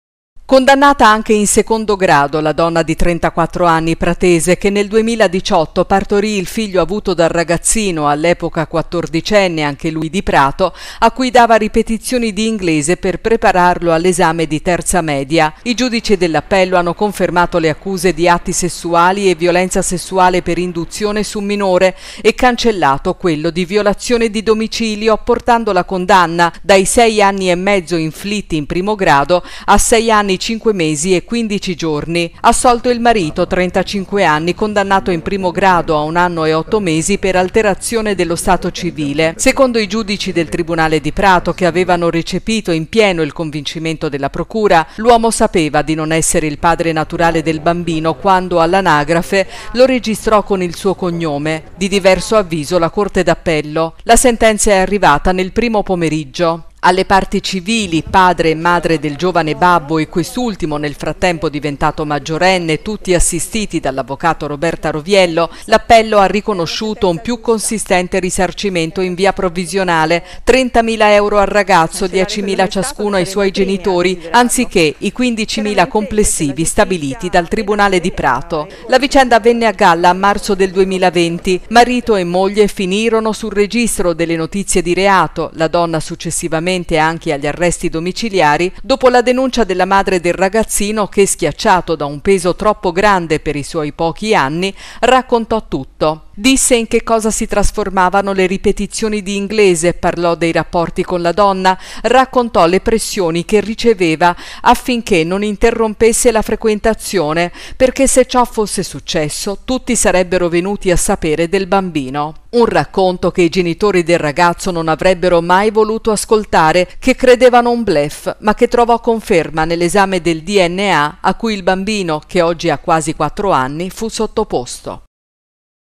2022-05-17 PRATO - FIGLIO DA 14ENNE, APPELLO CONDANNA LEI E ASSOLVE MARITO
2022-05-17 PRATO - FIGLIO DA 14ENNE, APPELLO CONDANNA LEI E ASSOLVE MARITO